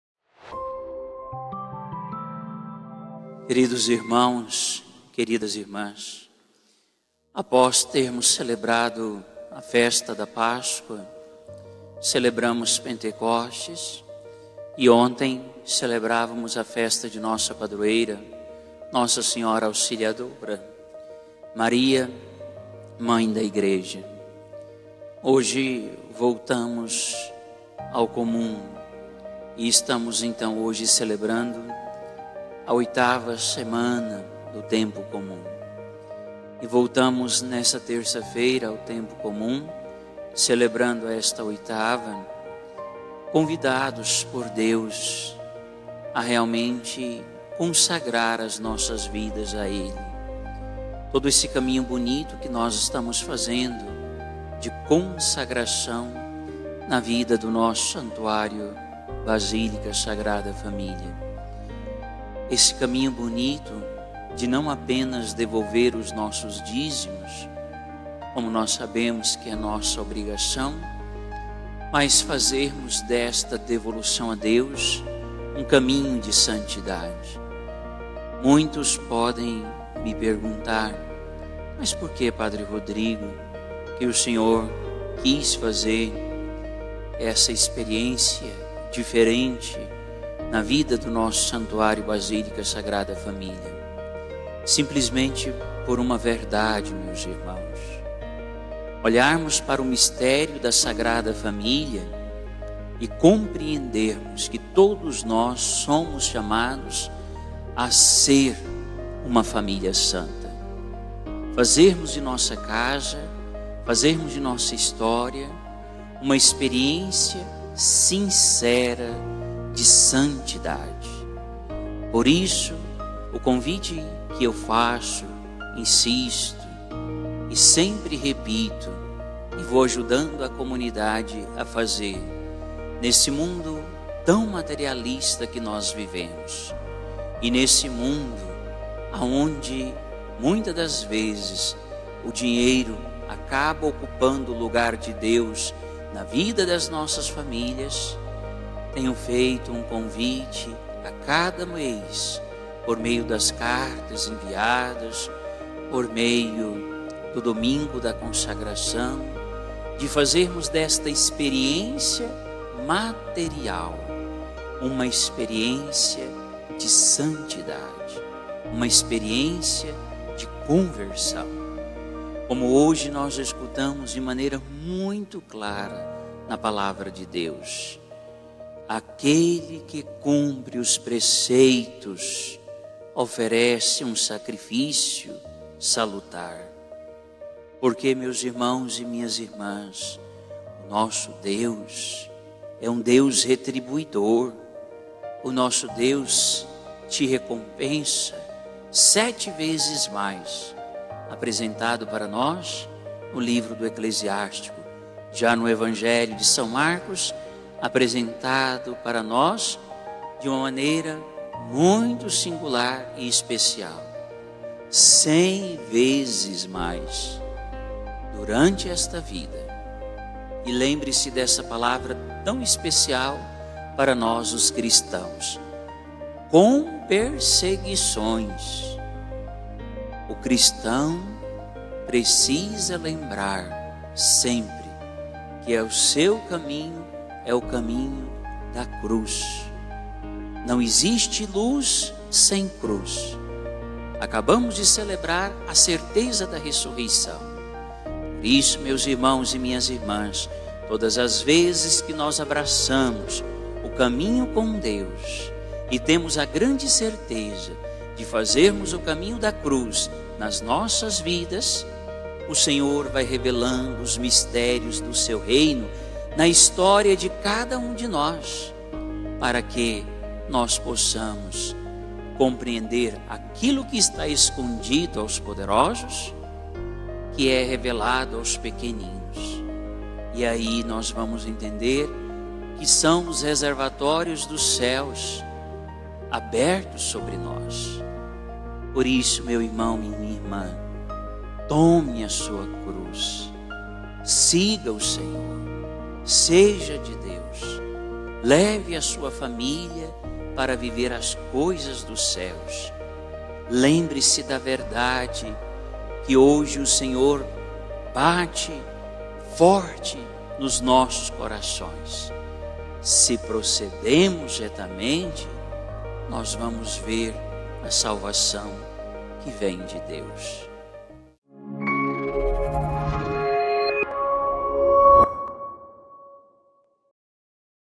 Homilia Diária - 25 de maio de 2021
Homilia Diária - Padre Rodrigo de Castro
Evangelho (Mc 10,28-31)
Santuário Basílica Sagrada Família

Santuário de Adoração Perpétua Sagrada Família.
Adoração à Santíssima Eucaristia, 24 horas online.